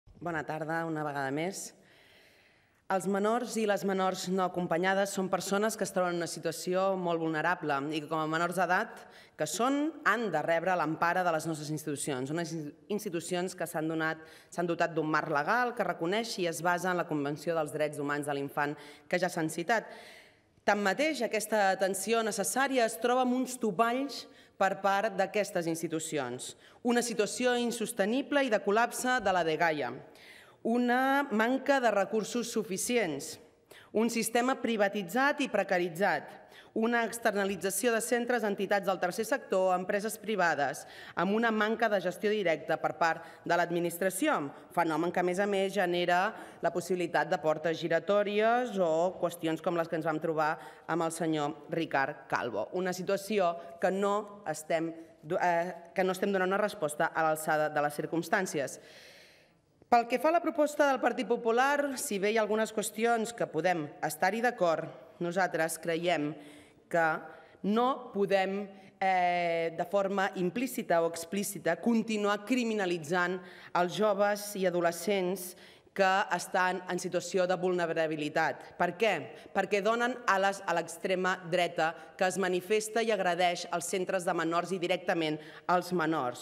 "Aquests joves han vingut per quedar-se i viure bé, i acollir-los bé"
25/07/2019 Natàlia Sànchez sobre els menors no acompanyats